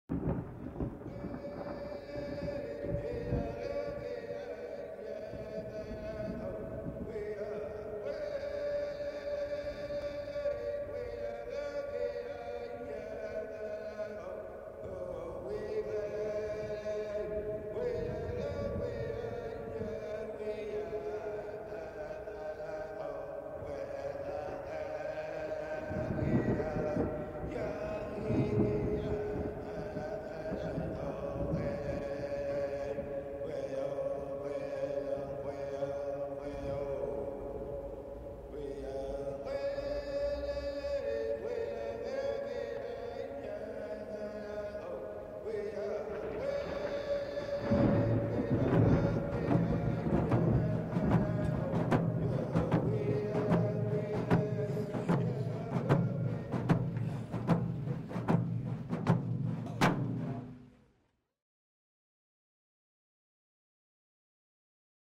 Yellowhead Tribal College Graduation - October 13, 2022
Graduation event at the Evario Centre located in Edmonton AB, for our class of 2022 at the Yellowhead Tribal College.